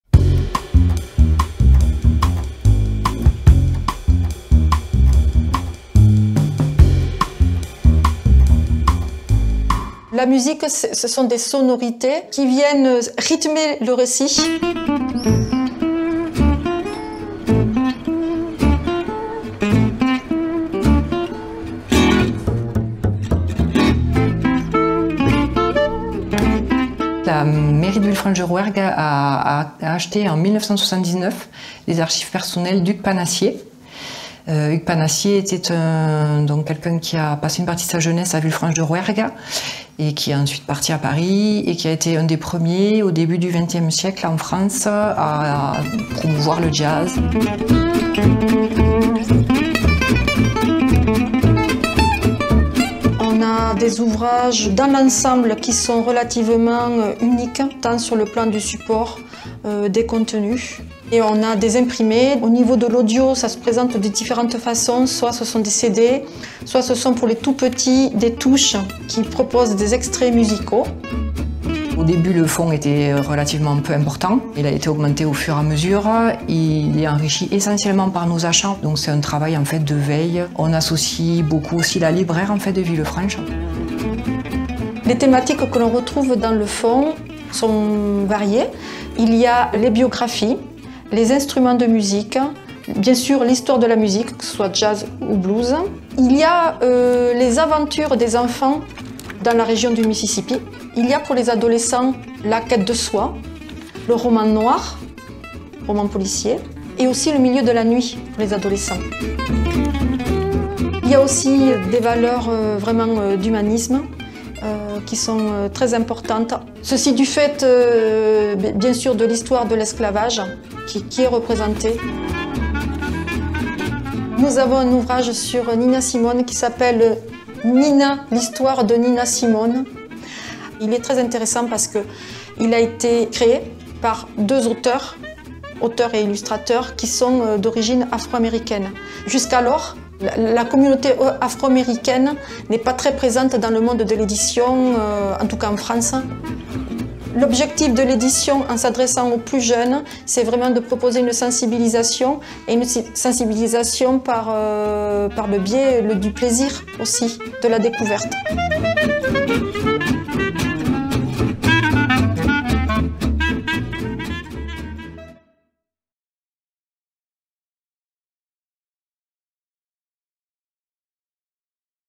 [TRAMPOLINE] Épisode #6 : Sur des notes de jazz (Villefranche-de-Rouergue)
TRAMPOLINE vous conduit cette fois-ci sur les rives du Mississipi, depuis la médiathèque de Villefranche-de-Rouergue en Aveyron.
La conservation partagée des collections jeunesse est une histoire de sensibilités, de rencontres et de lectures.
Si le jazz trouve son origine aux États-Unis, c'est Hugues Panassié qui l'a promu en France au début du 20e siècle. Ayant passé une partie de sa jeunesse à Villefranche-de-Rouergue, la Ville a acheté à sa mort ses archives personnelles. Comme l'explique Aline Bruel, responsable du secteur jeunesse et responsable du secteur documentaire adulte de la médiathèque municipale, de cette acquisition est née l'envie  de constituer et conserver une collection jeunesse dédiée au jazz et au blues qui s'enrichit au fil des ans.
Sa collaboratrice Isabelle Labat-Castaing, responsable du secteur musique et cinéma en précise la variété des supports (imprimés, CD, touches délivrant des sonorités) et la diversité des axes de conservation : biographies de musiciens, ouvrages sur les instruments, sur l'histoire de la musique de jazz et du blues, sur le milieu de la nuit, et bien entendu également sur l'histoire de l'esclavage. À travers son coup de coeur qu'elle nous partage ("Nina, L'histoire de Nina Simone" de Traci N. Todd et Christian Robinson, Didier jeunesse, 2021), elle envisage cette collection comme une sensibilisation à des valeurs d'humanisme, par le biais du plaisir et de la découverte.
Belle balade !
Production : Occitanie Livre & Lecture

avec le soutien du ministère de la Culture - Appel à projets Patrimoine écrit 2021
et en coopération avec le comité de pilotage du plan de conservation partagée des collections jeunesse

Réalisation : Extra Art, septembre 2022

Remerciements spéciaux à la Ville de Villefranche-de-Rouergue Aline Bruel et Isabelle Labat-Castaing